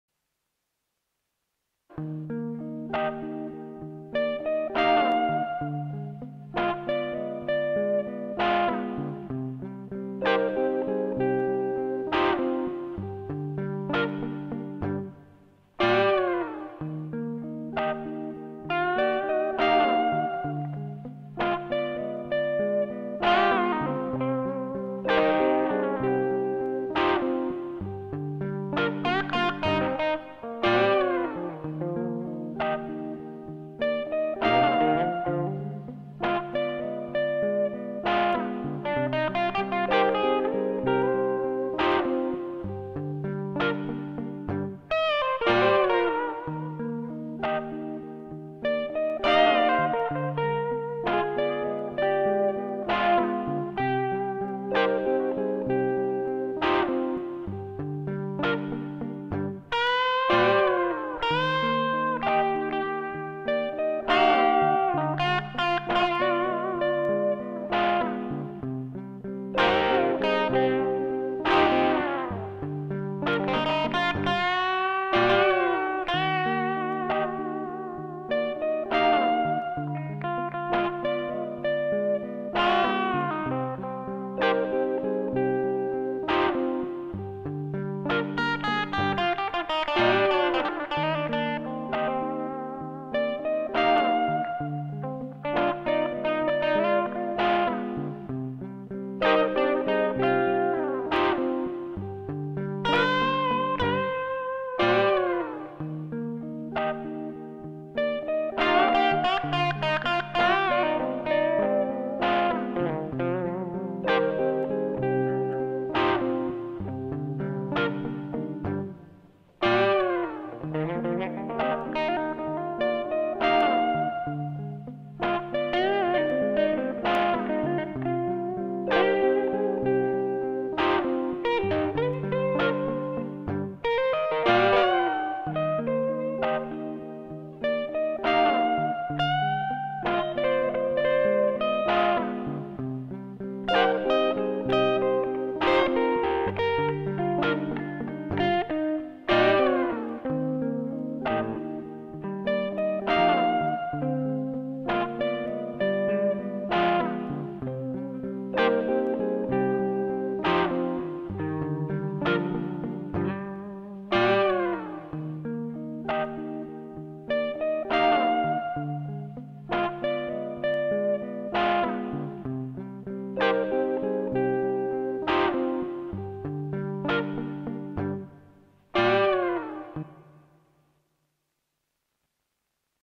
Mixolydian over 4 Chord Progression
In key of Eb and over the progression of
Eb D dim 7 to C minor then to G minor
nominally playing in C minor pentatonic
but moving freely over Bb Mixolydian.